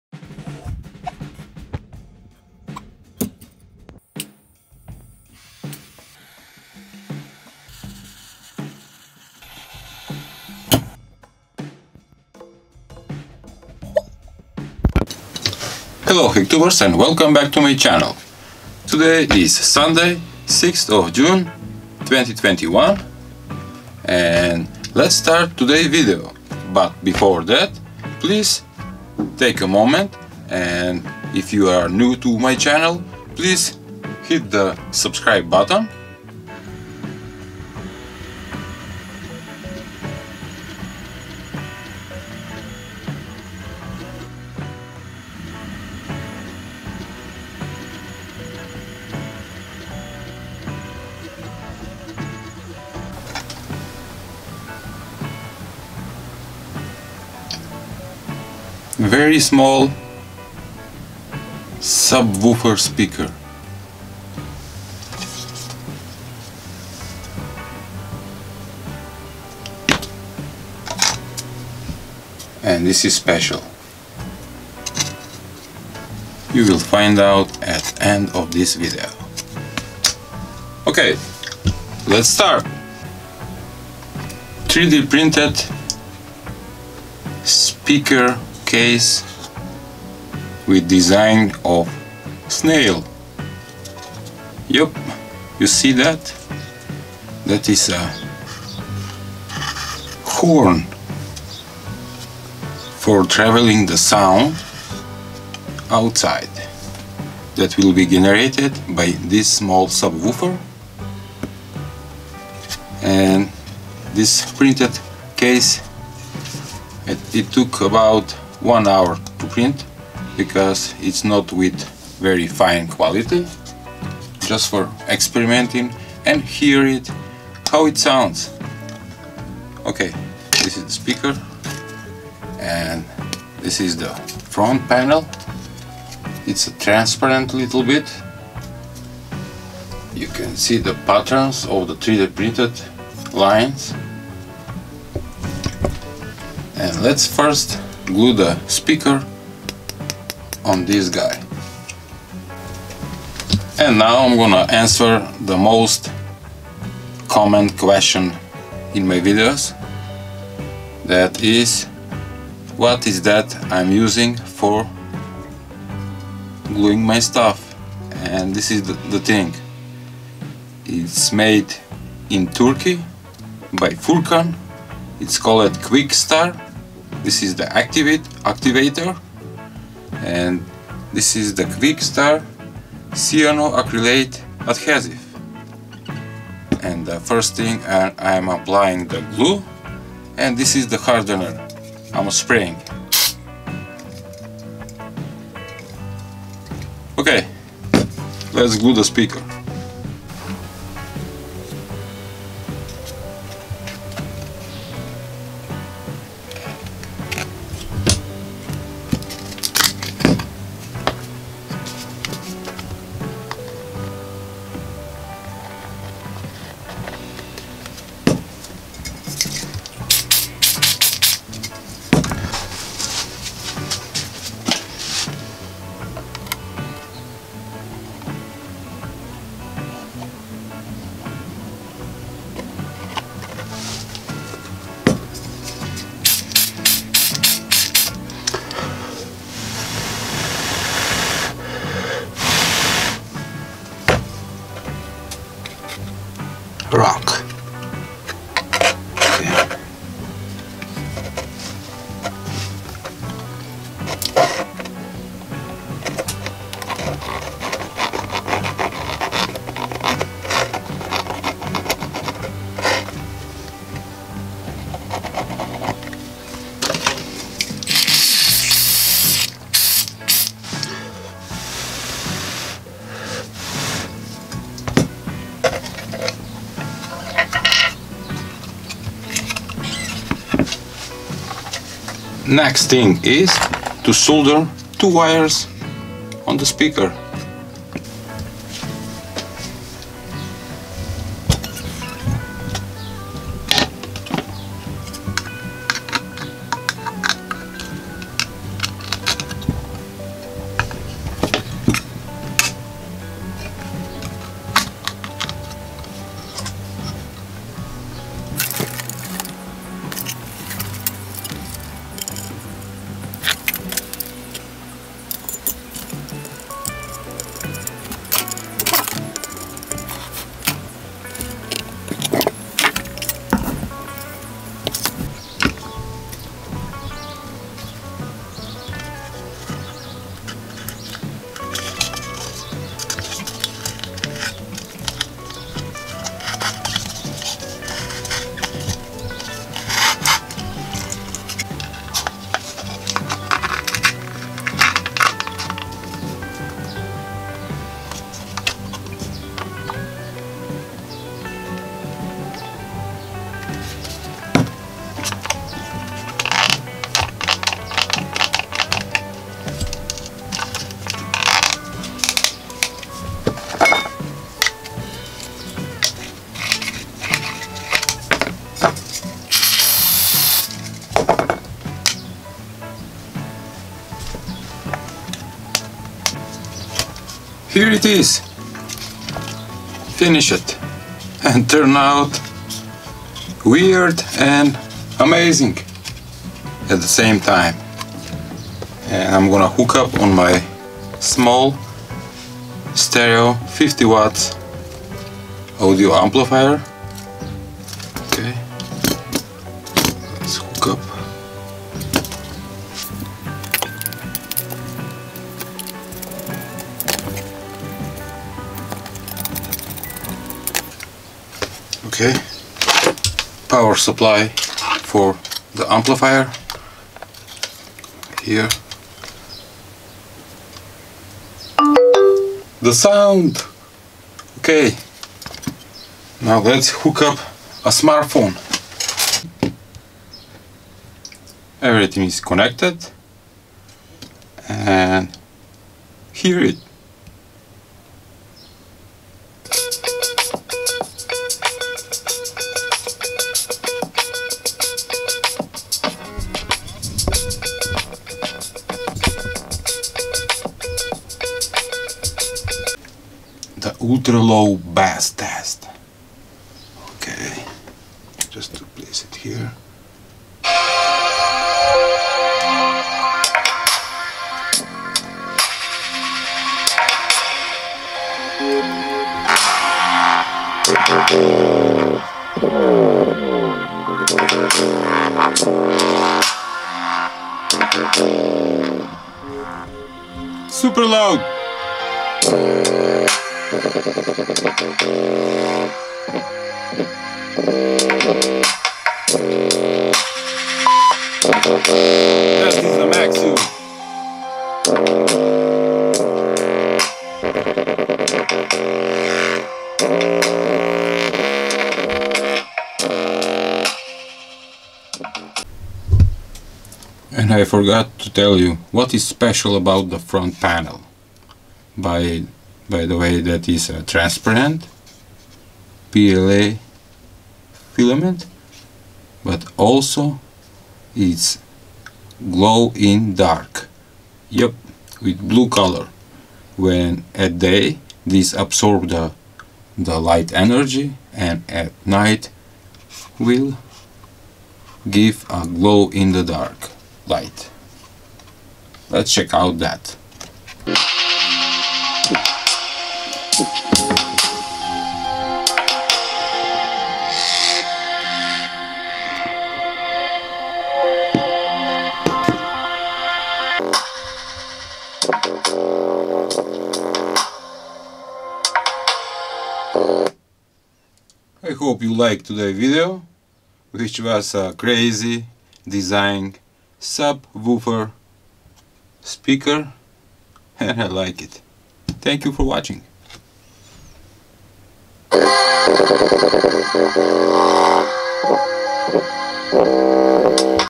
Making Snail Bass Speaker | Hear How it Sounds
I never think that mini subwoofer with snail design will be so amazing.